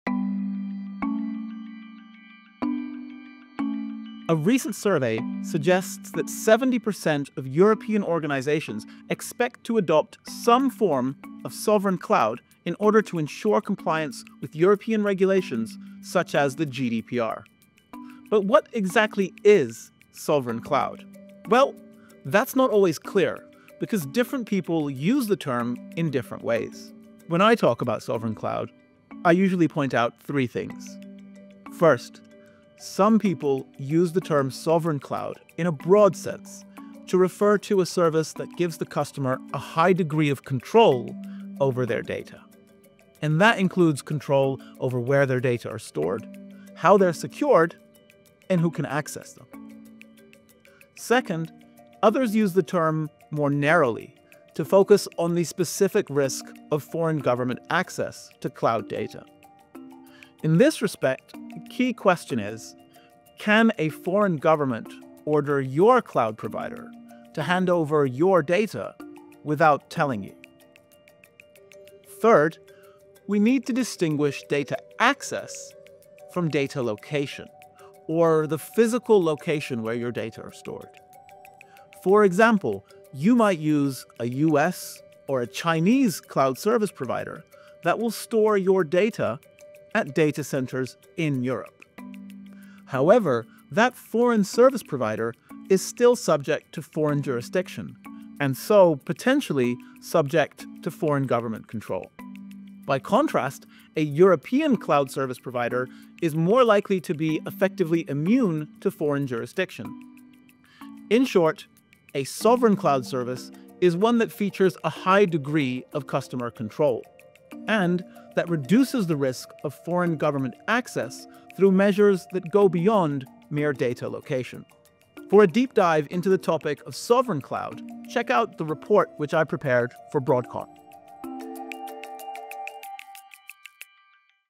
Explore sovereign cloud with Johan David Michels
A sovereign cloud service is one that features a high degree of customer control, and that reduces the risk of foreign government access through measures that go beyond mere data location. For a deep dive into sovereign cloud, see the report